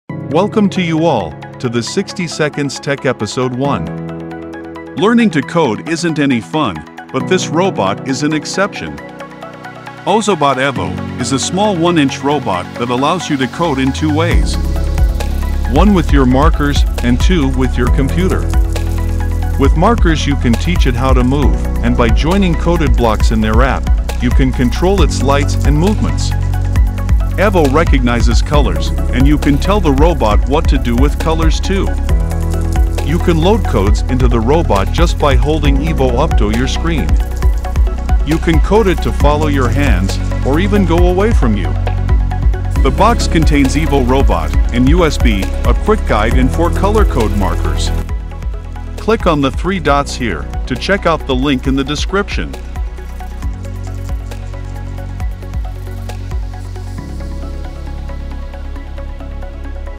Ozobot Evo : 1 Inch Small Fun Robot | 60 Seconds Tech Episode 1
Welcome to you all, to the 60 seconds tech episode1. Learning to code is one thing that kids hate. Leaning to code isn’t any fun, but this robot is an exception. Ozobot Evo, a small robot is an award winning coding robot, Evo is a small 1 inch robot which allows you to code in two ways. One with your markers, and two with your computer. With markers you can tech it how to move, and with the coded blocks in their app you can control movement and lights of the robot.

Our